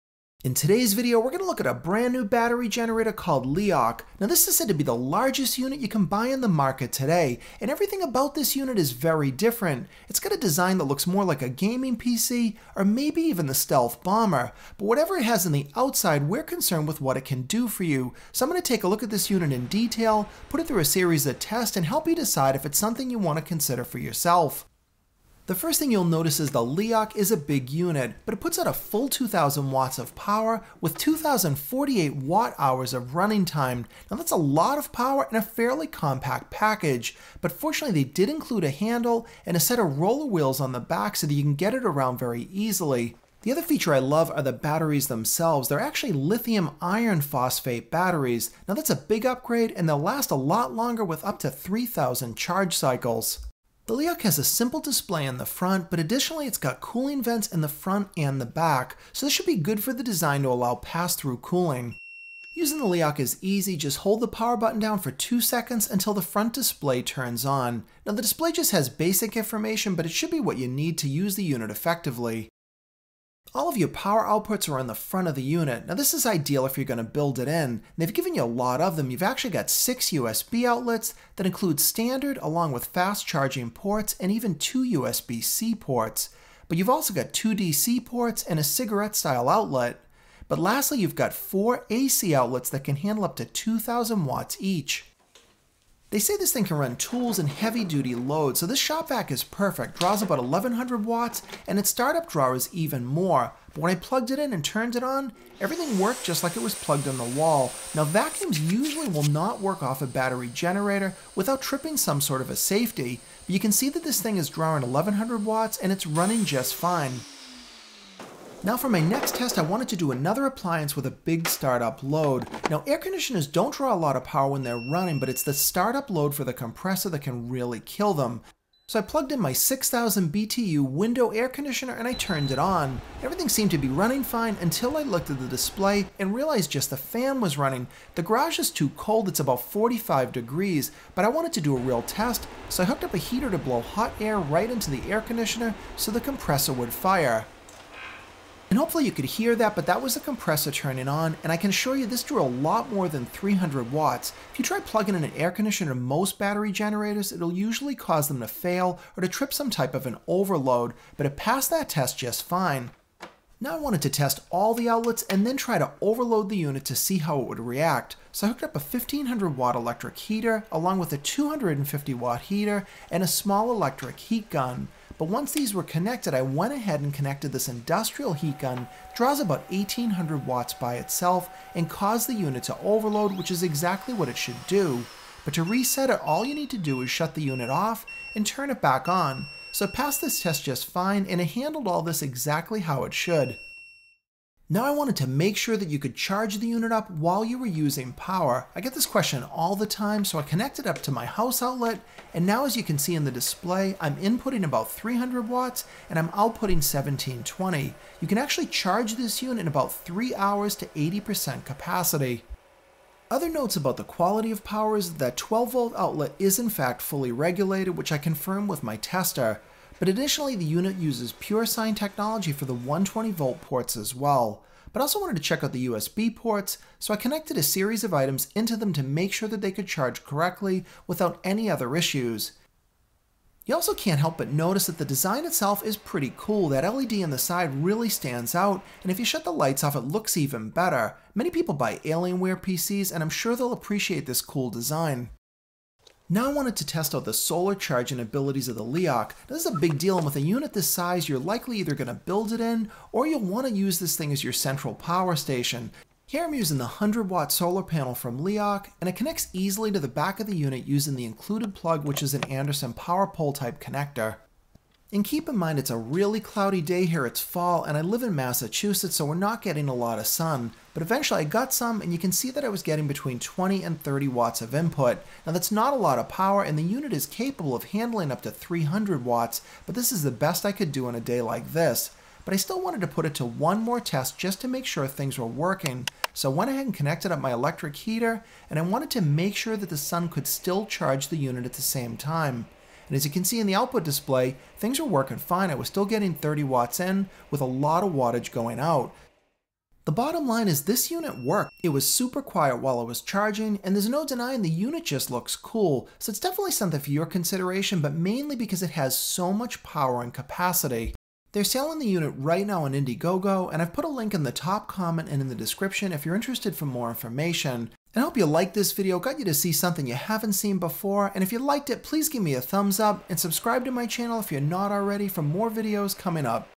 STEALTH GENERATOR $999 - LEOCH 2048 Next Generation Battery Power Station 2000 Watts! Review
The LEOCH 2048 Battery Power Station is a STEALTH GENERATOR that looks like it came from the pentagon or maybe Alienware. See my review as I put it through all the tests to help you decide if it's right for you.